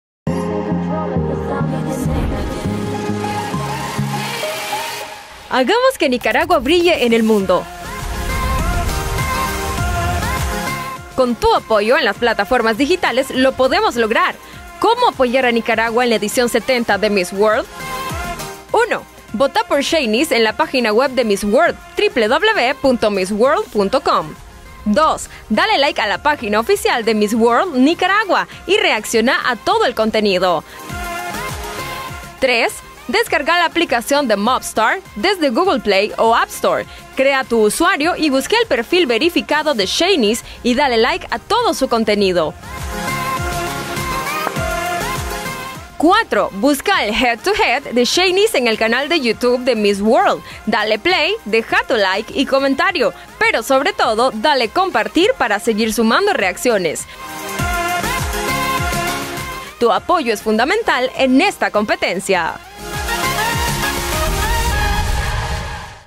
Miss Mundo 2021: ¿Cómo votar por Sheynnis Palacios? ¡Vota aquí!
A Sheynnis Palacios le llegó el momento de partir a Puerto Rico y demostrar sus habilidades en la pasarela internacional de Miss Mundo 2020.